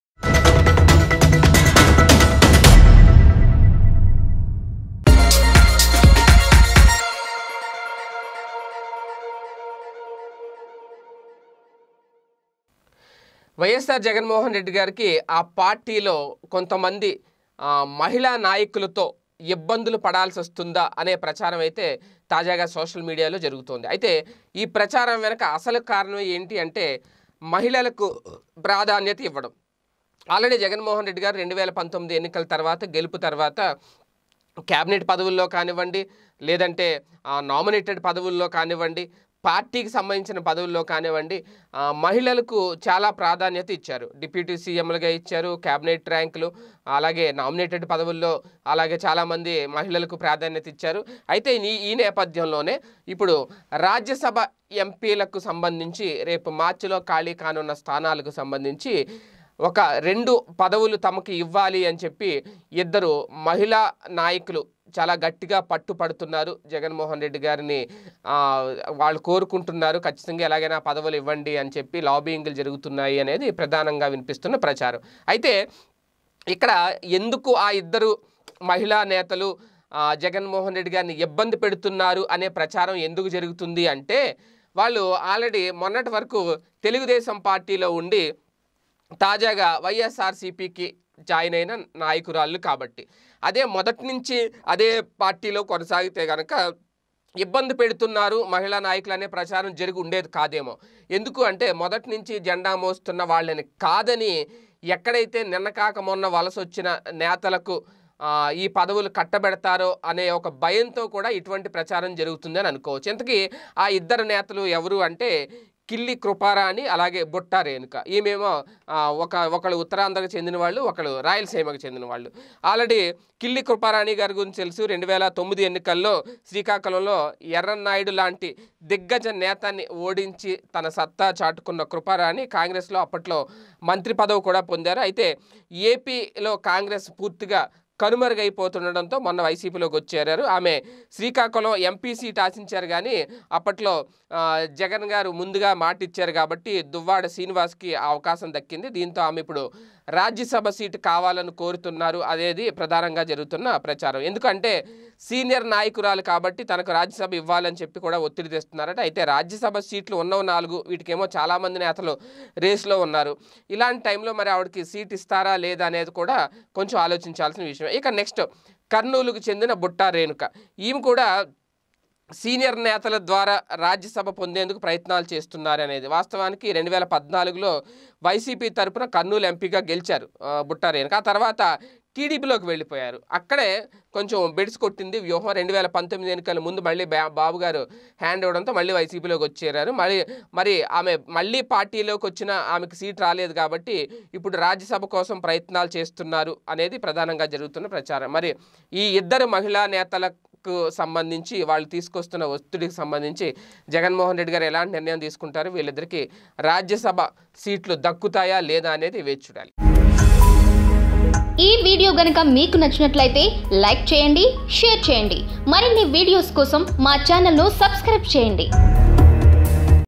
YSRCP Lady Leaders Force On CM Jagan | ఆ లేడీ లీడర్స్ తో జగన్ కు ఇబ్బందే..?
ఆ లేడీ లీడర్స్ తో జగన్ కు ఇబ్బందే..? | YSRCP Lady Leaders Force On CM Jagan | PDTV News